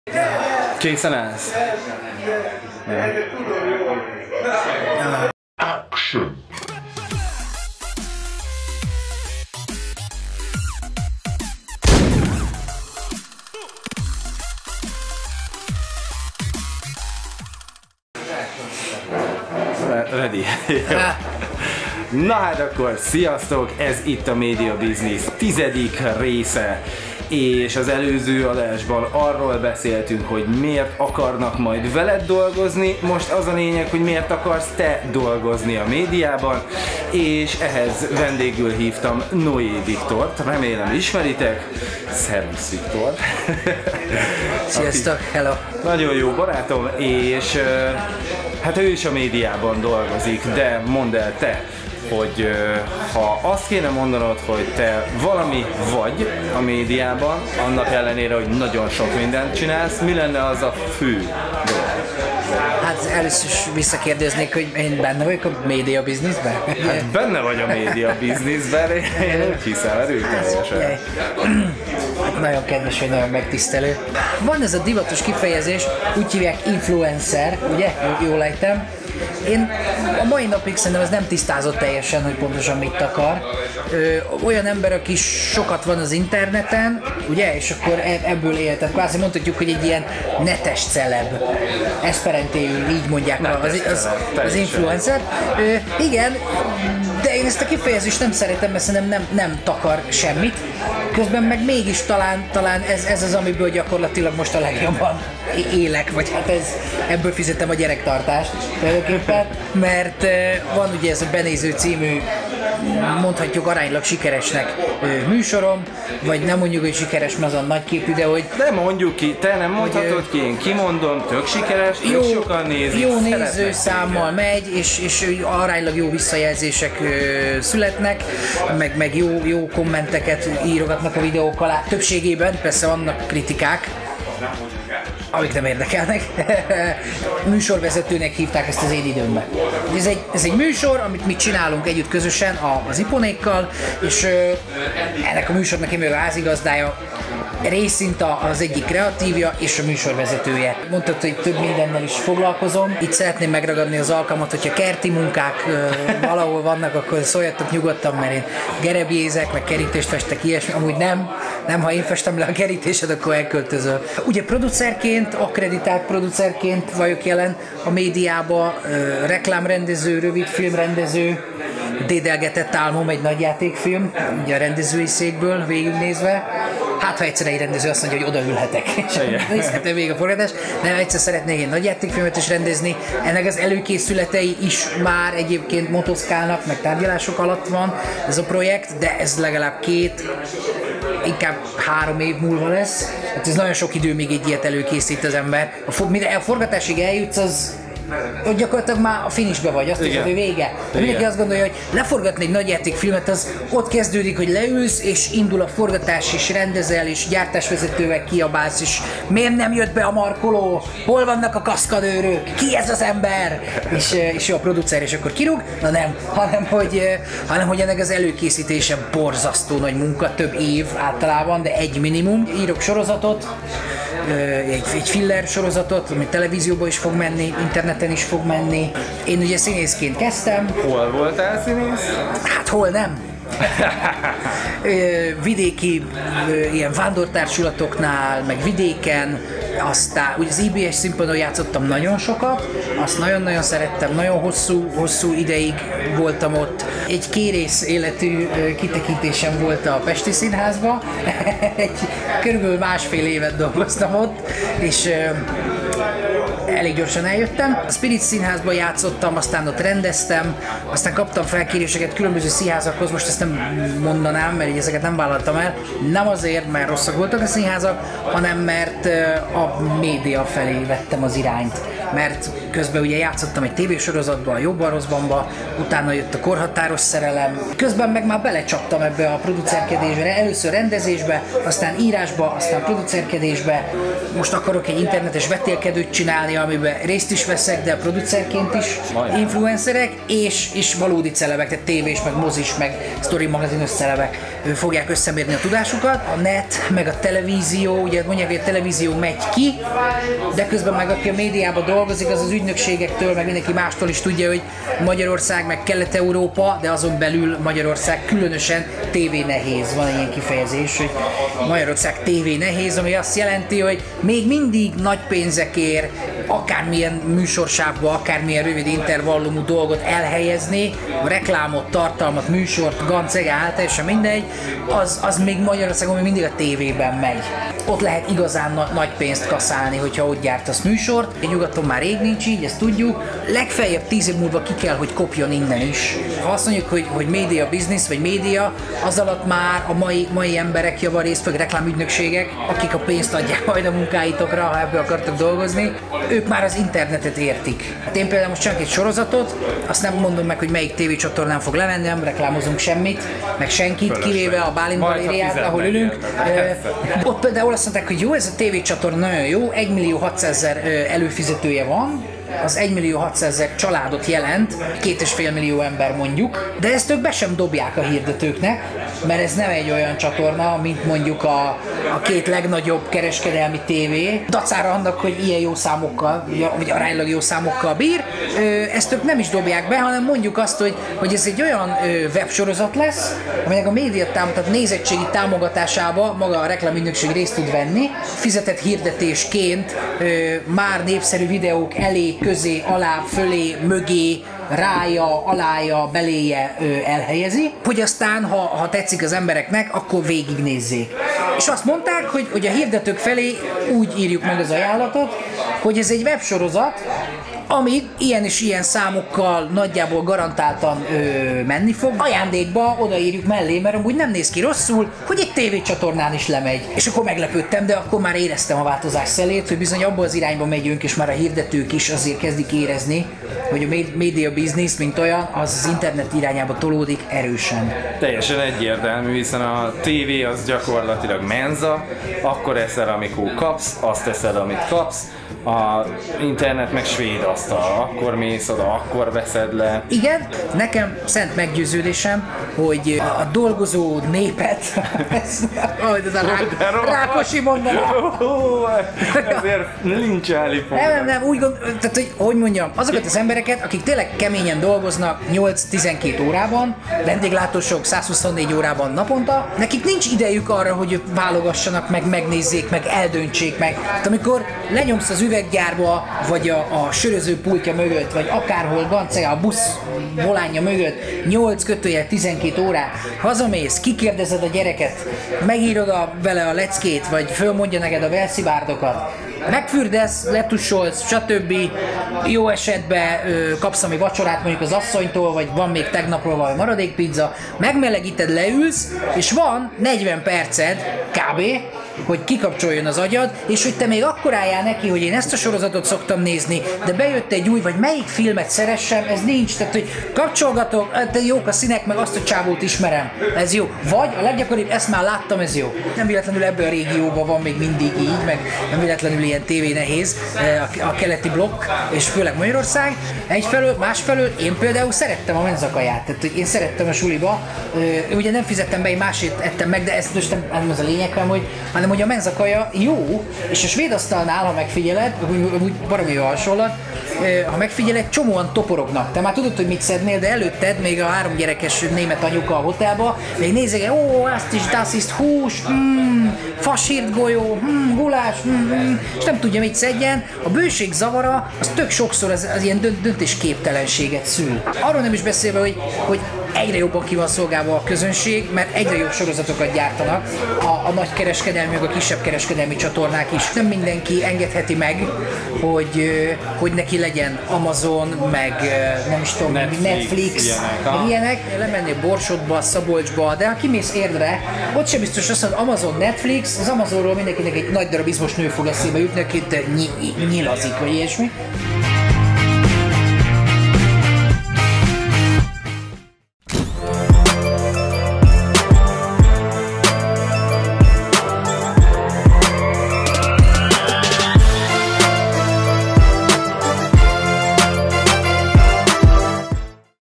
Média biznisz 10 rész 2:1 vendég: Noé Viktor - Bálint galéria és kávézó
Médiabiznisz 10.rész

Vendég: Noé Viktor
helyszín: Bálint galéria és kávézó
műsorvezető: Orbán Gergely

A Médiabiznisz sorozat azoknak szól, akik a médiában dolgoznak, vagy szeretnének dolgozni. Célunk, hogy a lehető legtöbb hasznos információt juttassuk el nézőinknek, amik a filmes iskolákból kimaradtak. Szeretnénk megtanítani, hogyan lehet ma Magyarországon érvényesülni a médiaszakmában.

A mostani részben Noé Viktor producer meséli el nekünk, ő miért, és hogyan vágott bele a médiaszakmába!